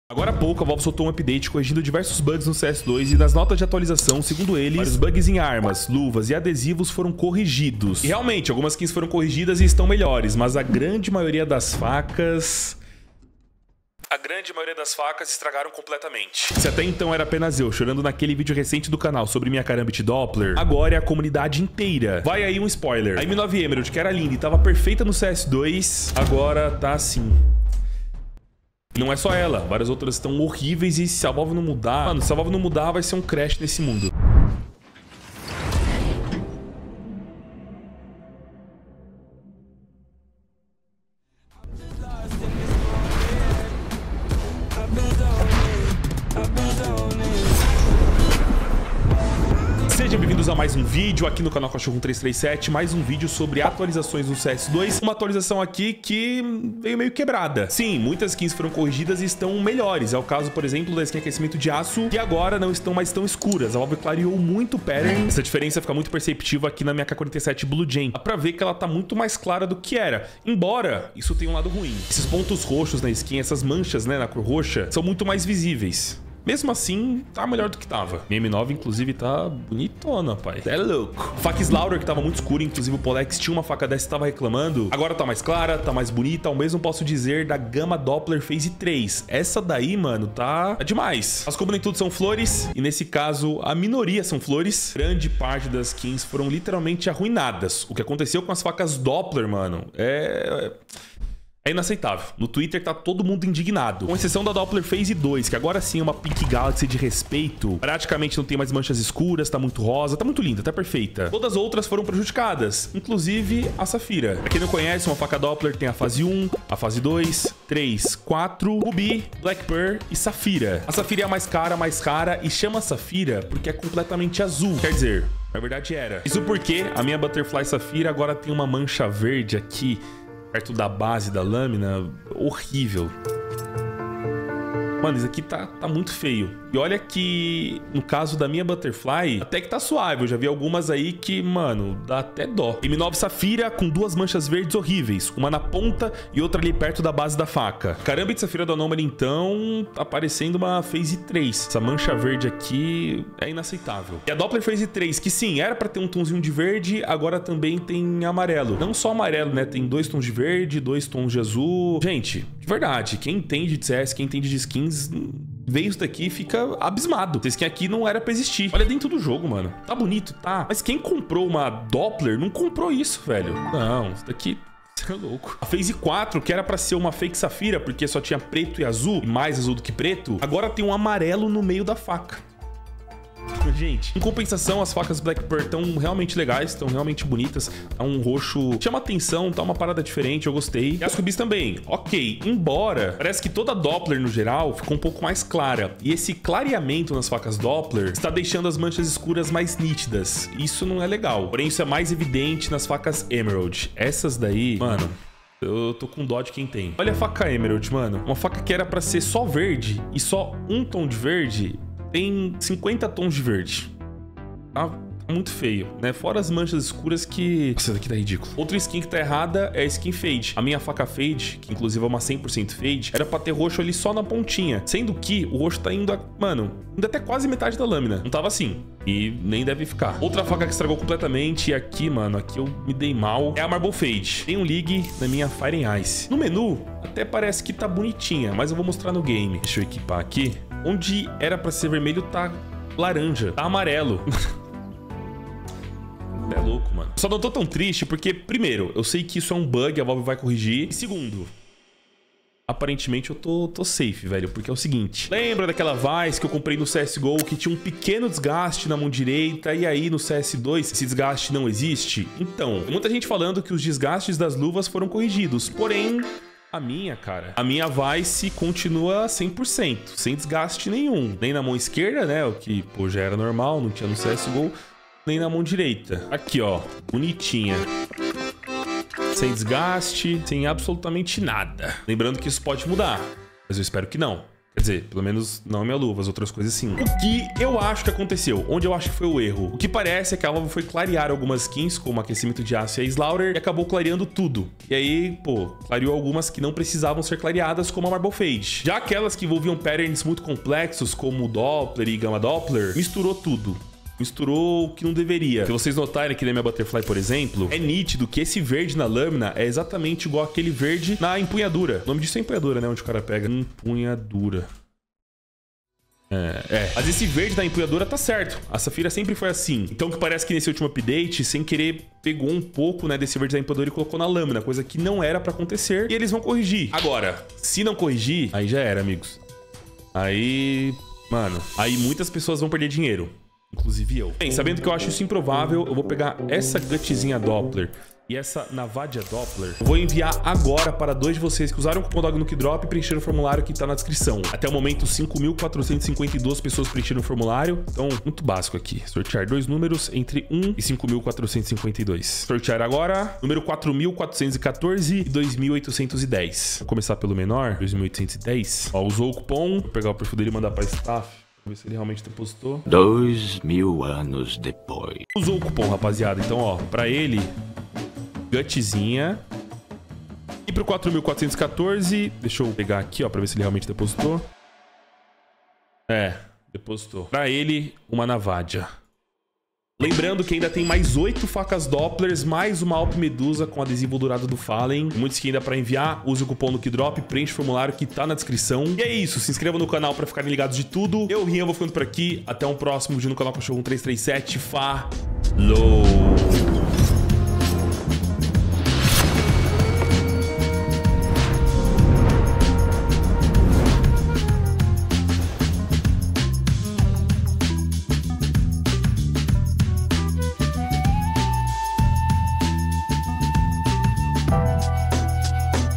F, agora acabou pra geral... VALVE ACABOU COM AS FACAS DO CS 2
▸Troque suas skins |​​​​​​​​​​ Cupom: DOG

A Valve soltou um update ontem de CS 2 que corrigiu diversos bugs de lag, desempenho, e outras coisas, mas acabou mudando texturas de diversas skins que não deveriam...